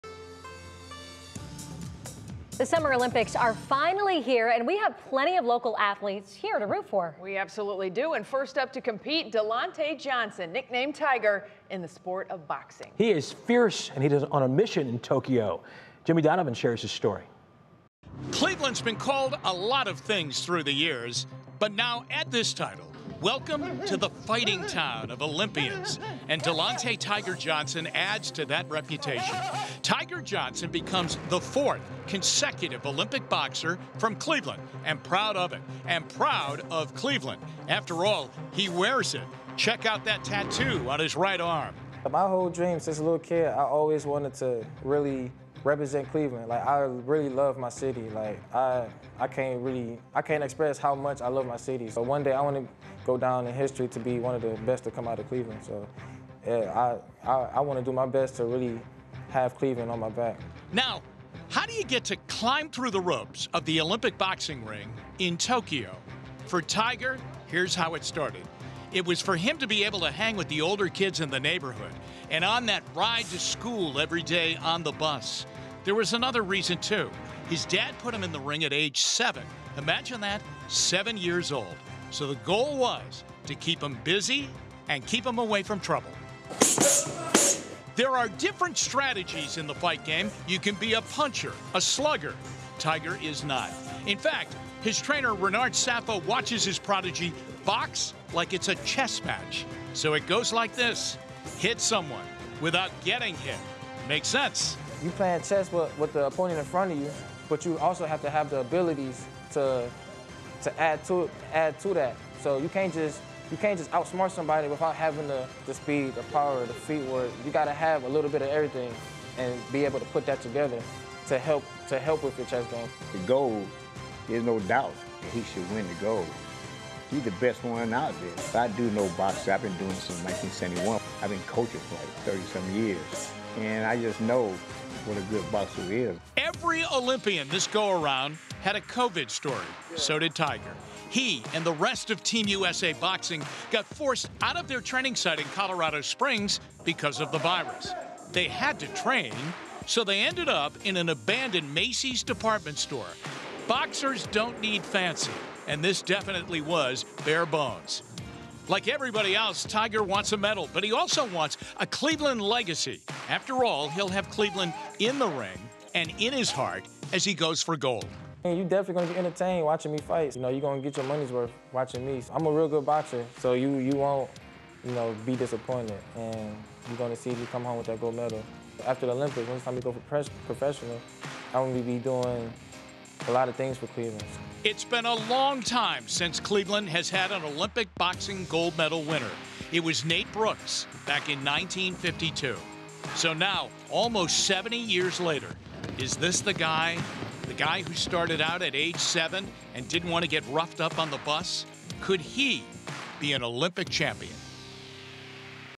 Cleveland boxer Delante Johnson hoping for success at Olympics
Delante Johnson is about to realize his Olympic dream.

The Cleveland native known as "Tiger" will compete in the men's welterweight boxing competition starting Saturday. He is attempting to become the first Clevelander since Nate Brooks in 1952 to win a gold medal in boxing, and is proud to represent the city on the world stage.

"I really love my city," Johnson told 3News. "I want to go down in history to be one of the best to come out of Cleveland."